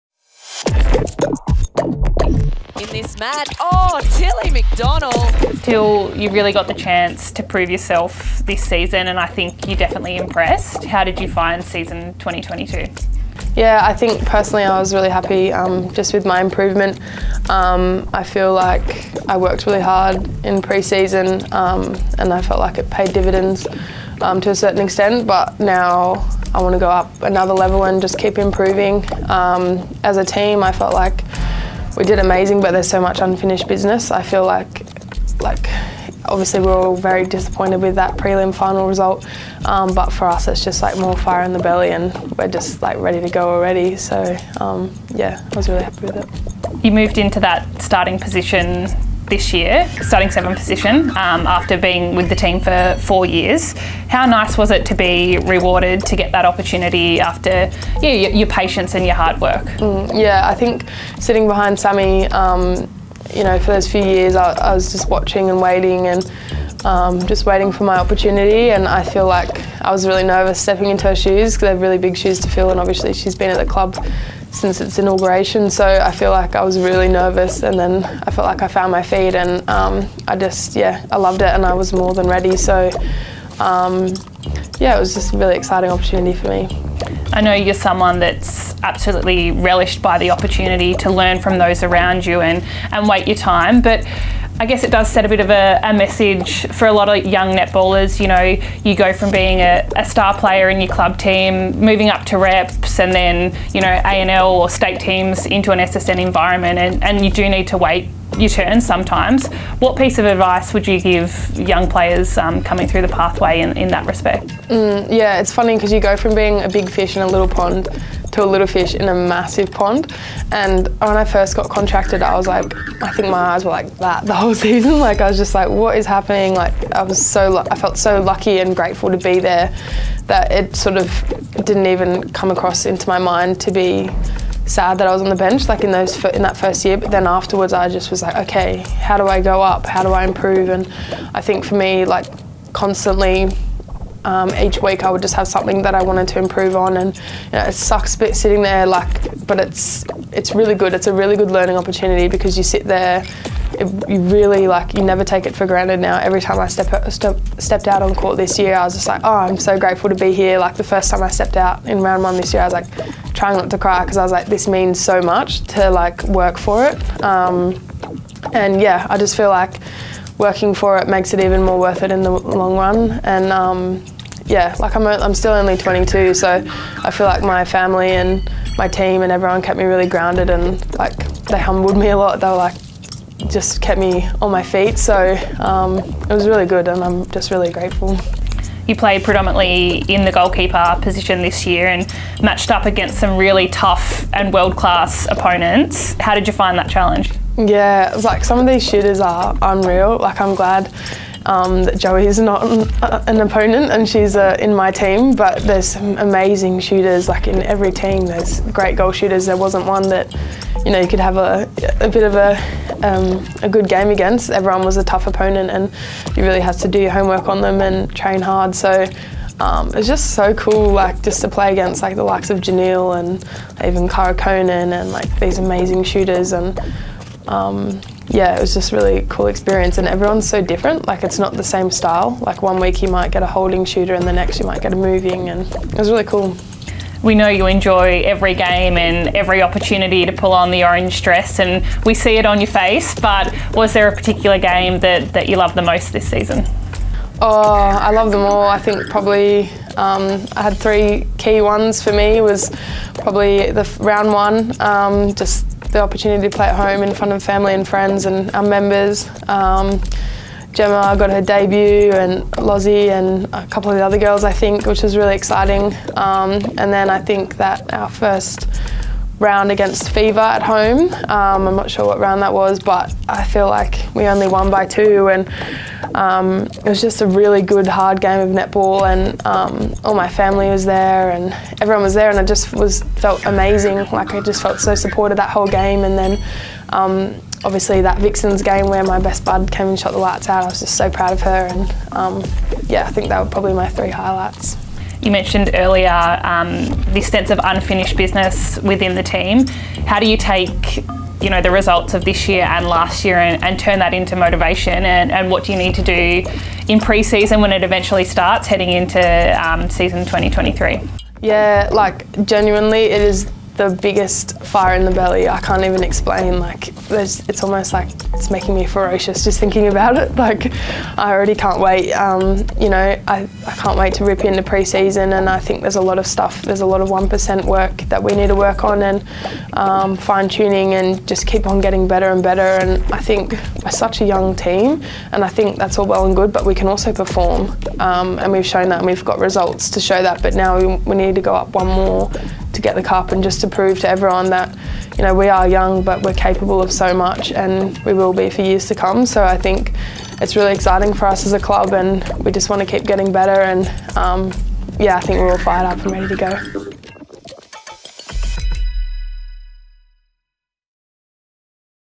GIANTS Netball Season Reflection: Tilly McDonell
It was a season full of challenges for rising star Tilly McDonell.